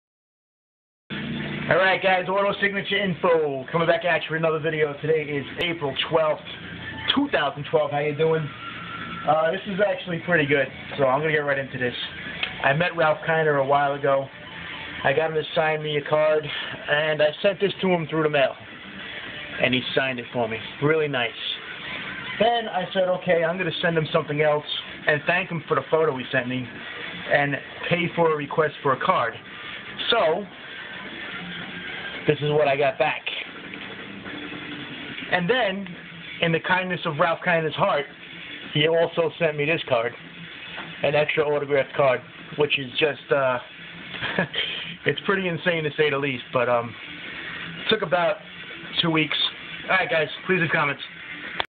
4/12/12 AWESOME BASEBALL HOF TTM 1950 CARD + EXTRA
RALPH KINER...PAID FOR REQUEST...ALSO ADDED AN EXTRA ON HIS OWN...KINER IS NOT AN EASY RETURN FOR SOME REASON